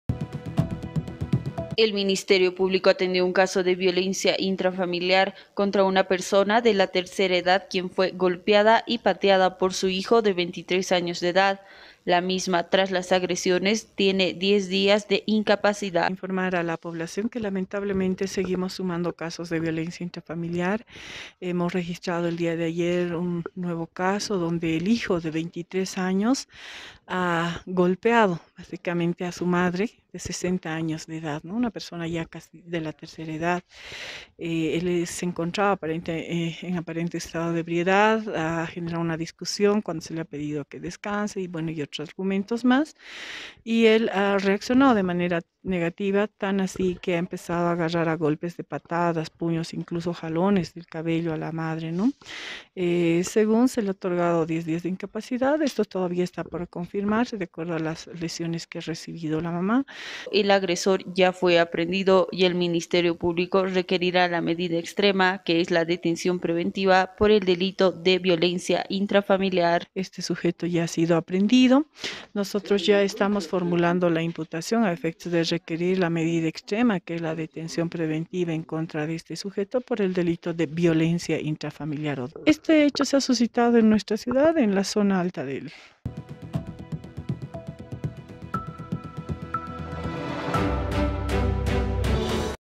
MUJER DE LA TERCERA EDAD ES AGREDIDA POR SU HIJO
El Ministerio Público informó acerca de un caso de violencia intrafamiliar contra una mujer de la tercera edad por parte de su hijo de 23 años, quien agredió a su madre con golpes y patadas.